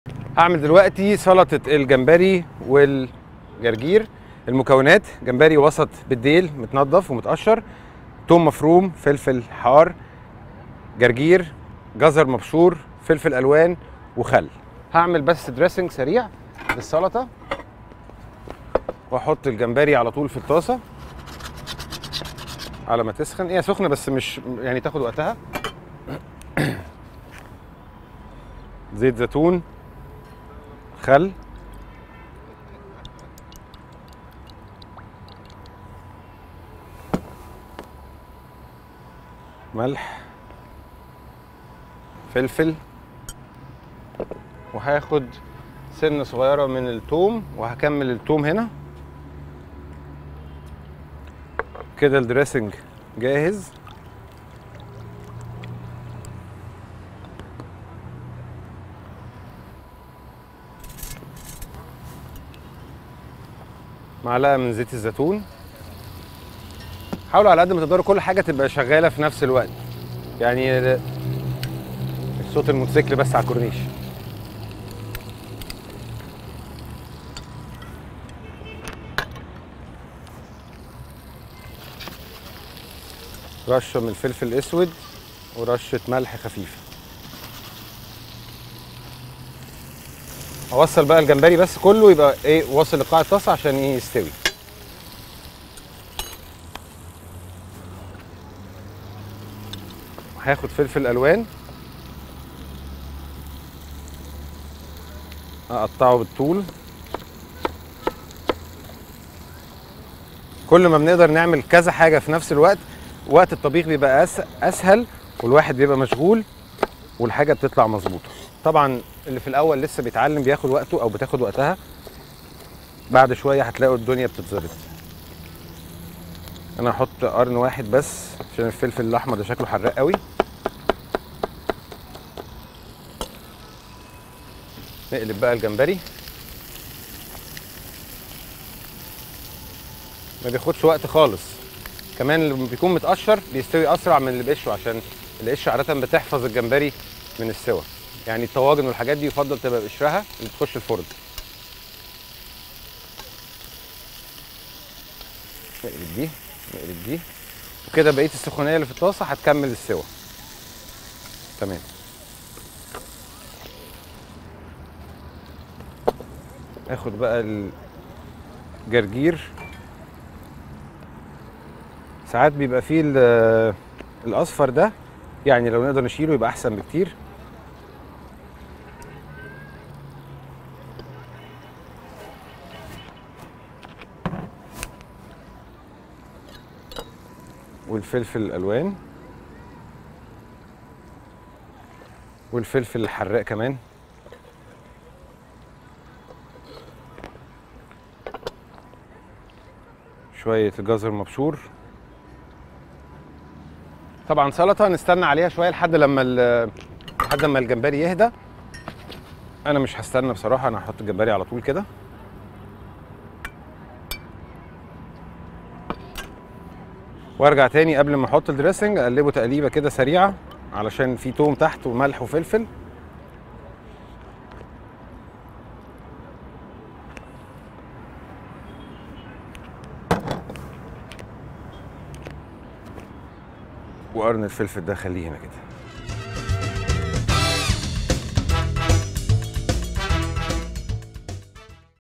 سلطة جمبري بالجرجير | هشام السيد
طريقة عمل سلطة جمبري بالجرجير  مع هشام السيد في شبكة وصنارة